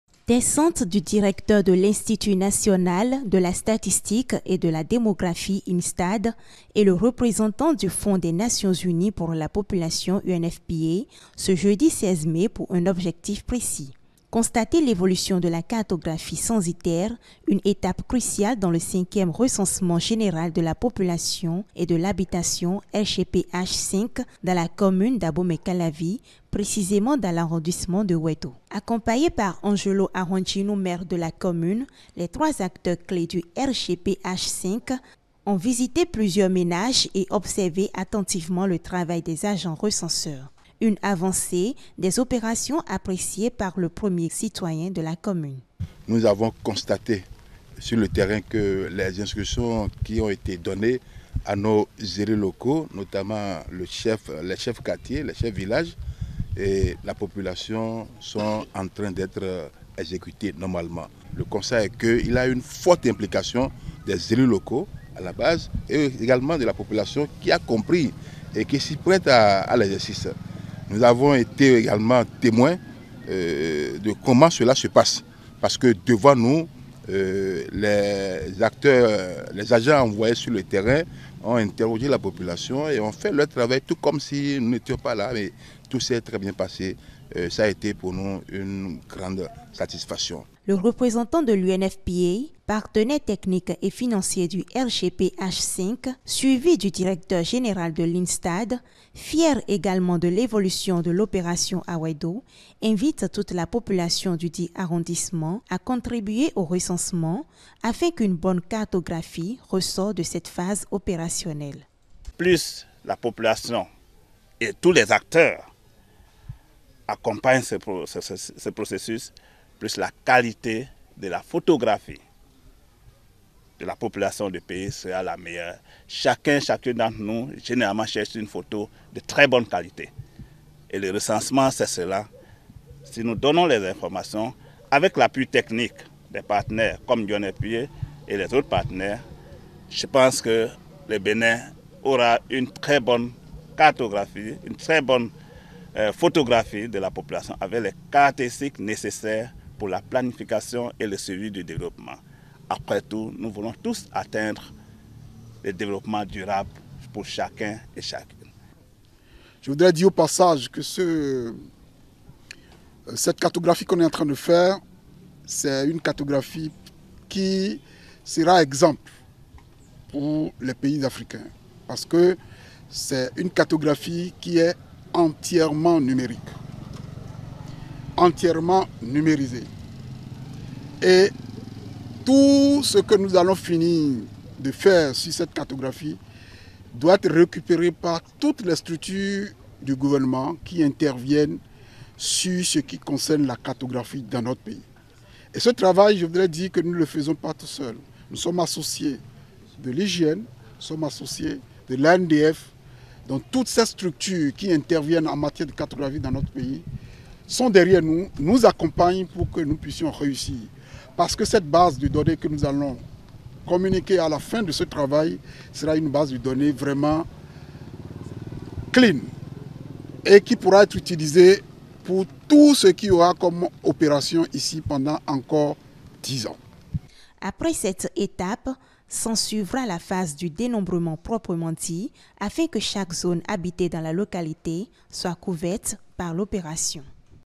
📍 Déroulement de la cartographie censitaire RGPH5 à Ouèdo : l’INSTAD et l'UNFPA s’enquièrent de...
Déroulement de la cartographie censitaire RGPH5 à Ouèdo :  l’INSTAD et l'UNFPA s’enquièrent de l’évolution de l’opération.